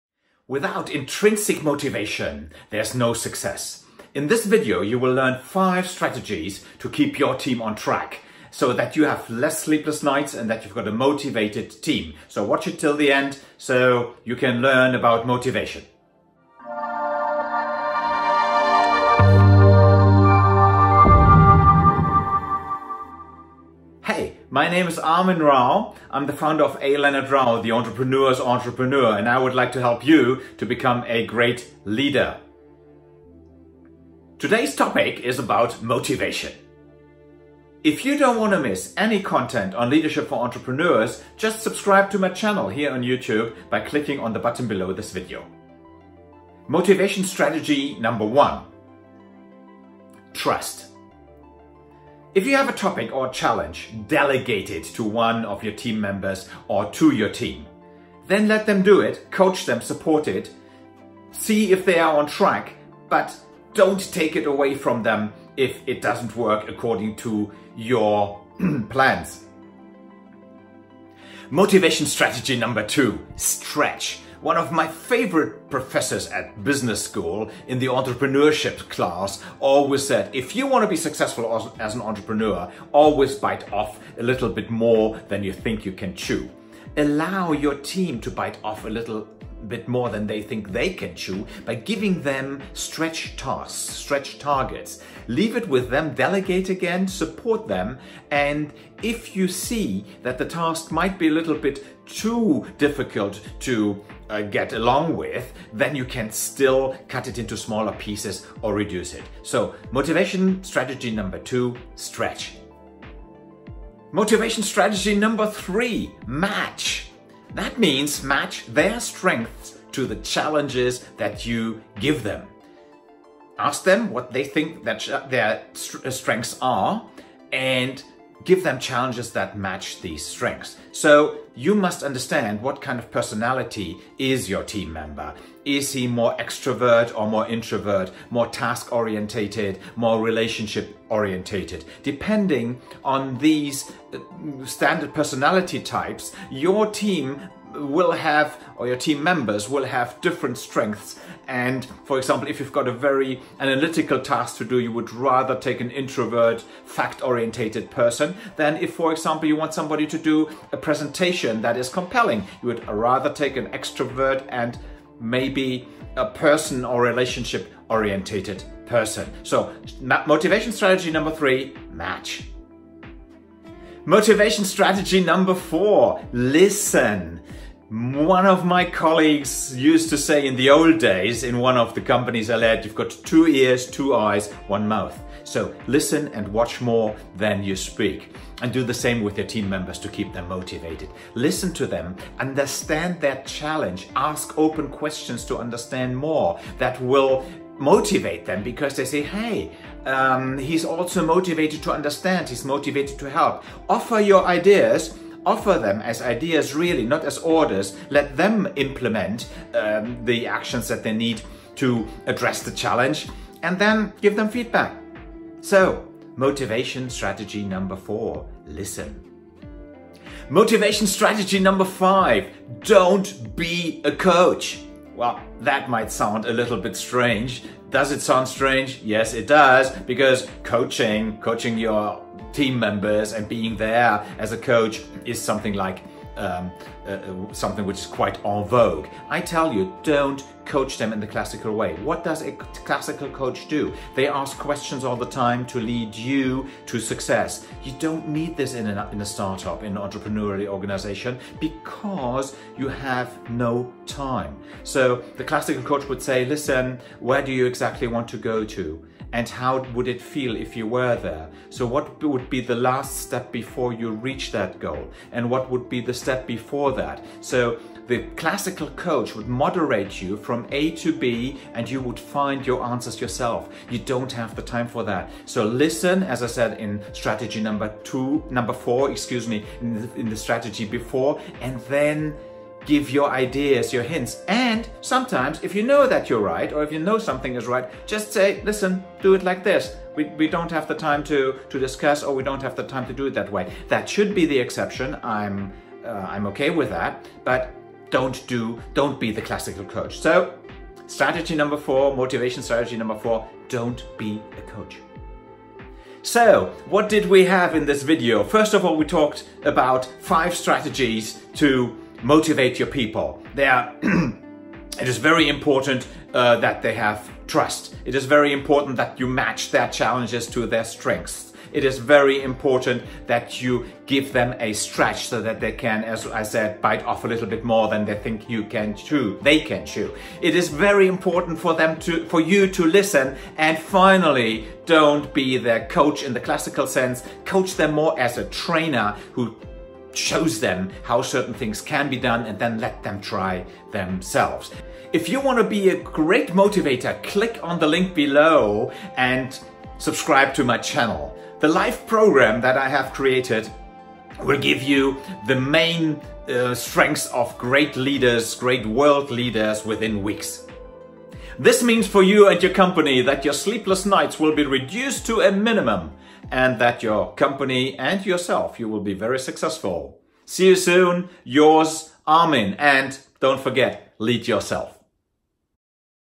5 Motivation Strategies in 2021
Employee motivation is one of the essential tasks for you as an entrepreneur and leader. Your team has to follow you and your vision. It must work for you and your success. Because your success is theirs. In this video, I show you how to motivate your team members.

Watch: youtu.be/Tv6WImqSuxA